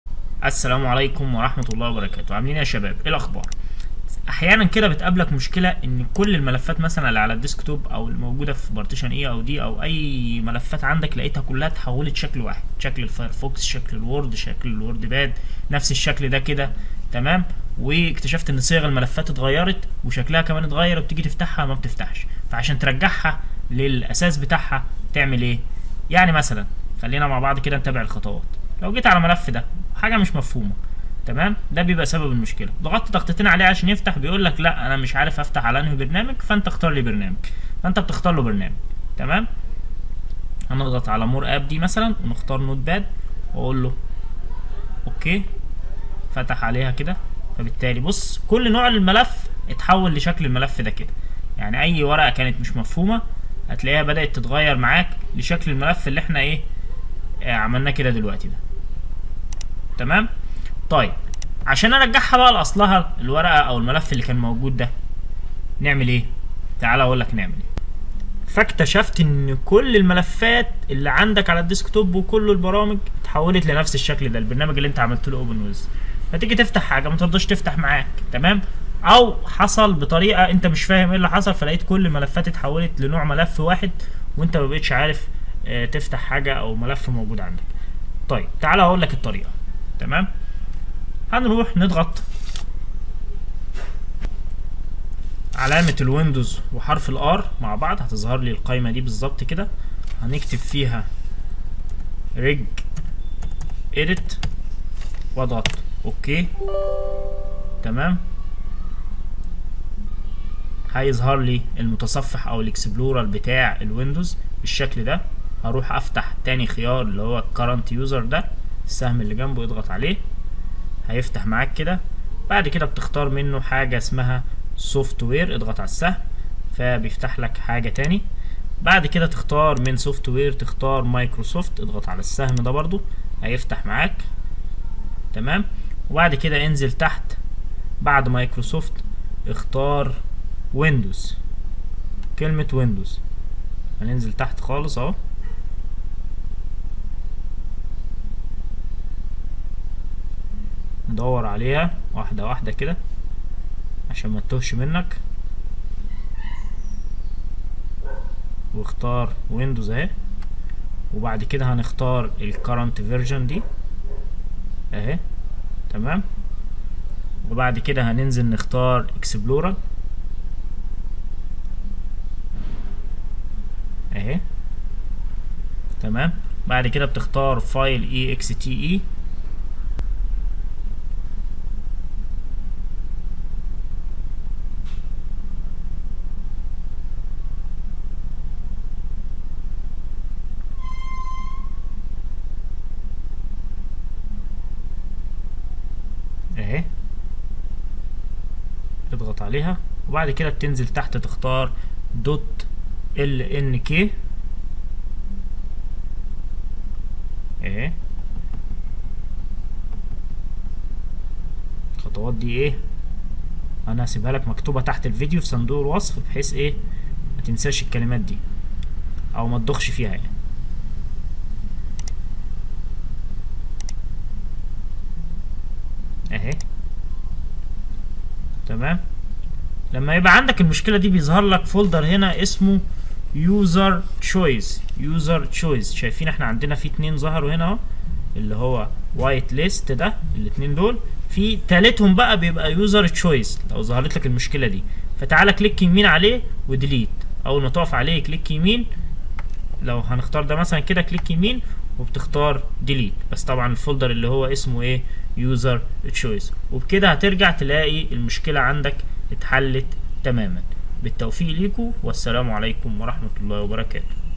حل مشكلة ظهور جميع البرامج بنفس الايقونة واستعادة شكل الايقونات الاصلية
regedit
HKEY_CURRENT_USER
SOFTWARE
MICROSOFT
WINDOWS
CURRENTVERSSION
EXPLORER
FILETEXTE
.LNK
USER CHOICE